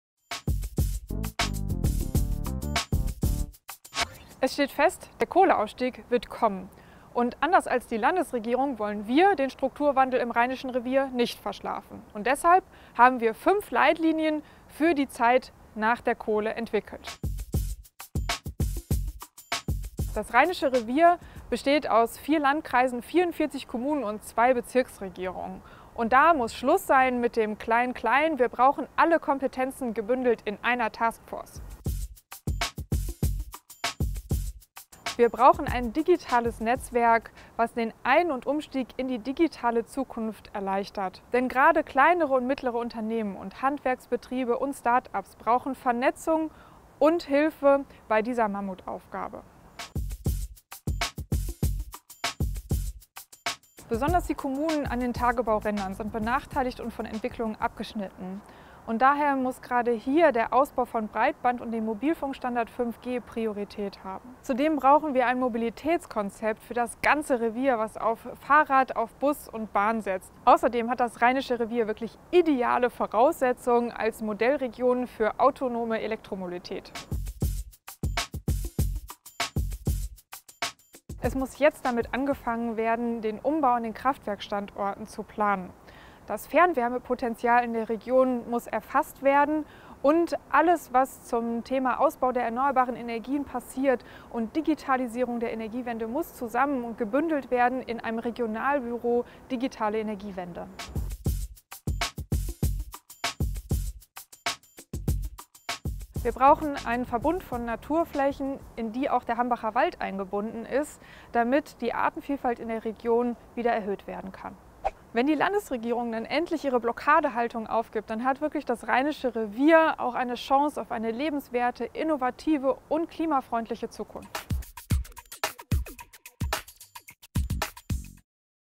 Wibke Brems: Leitlinien für ein zukunftsfähiges Rheinisches Revier
Wibke Brems hat heute unseren Antrag für eine nachhaltige Umgestaltung des Rheinischen Reviers eingebracht. Die Menschen in der Region brauchen eine Zukunftsperspektive und einen sozialverträglichen Ausstieg aus der Braunkohle und auch die Wirtschaft braucht Planungssicherheit, um sich zukunftsfähig aufstellen zu können. Dazu haben wir konkrete Leitlinien entwickelt. Mehr dazu im Video: